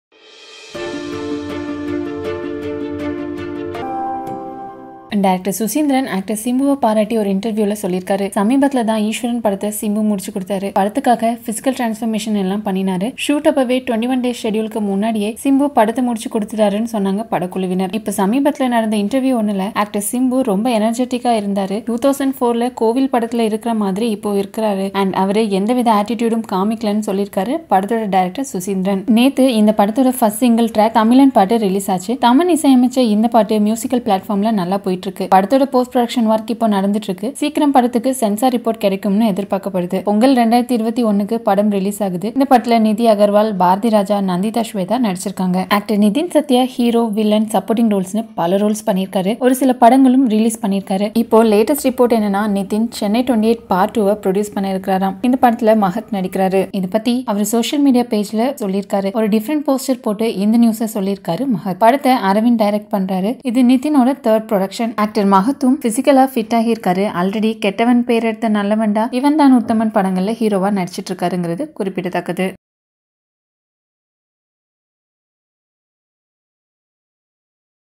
Director Suseenthiran praises Simbu | Eeswaran | Nithin Sathya to produce film featuring Mahat
Director Suseenthiran praises Simbu for his co operation in Eeswaran | Nithin Sathya to produce film featuring Mahat